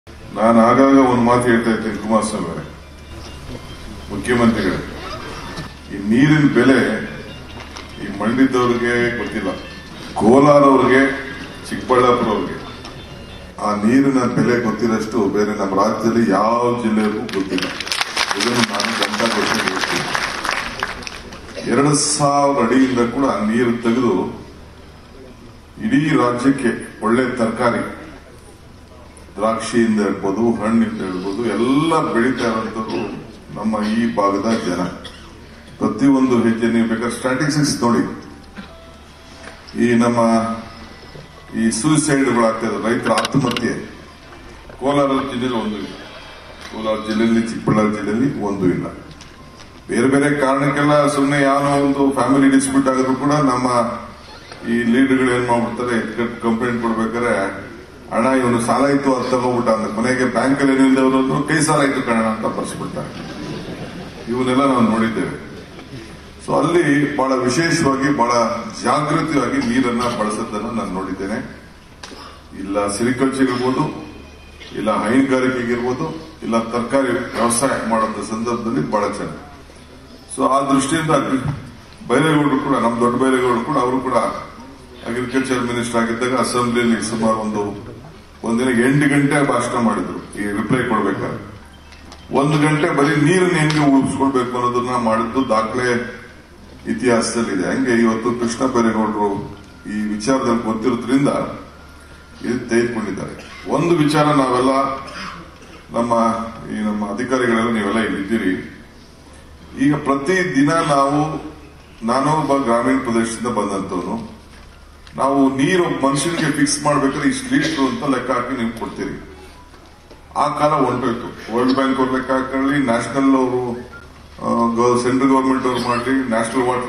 ಮುಖ್ಯಮಂತ್ರಿಗಳೇ ನೀರಿನ ಬೆಲೆ ಮಂಡ್ಯದವರಿಗೆ ಗೊತ್ತಿಲ್ಲ | DK Shivakumar | TV5 Kannada
ಮುಖ್ಯಮಂತ್ರಿಗಳೇ ನೀರಿನ ಬೆಲೆ ಮಂಡ್ಯದವರಿಗೆ ಗೊತ್ತಿಲ್ಲ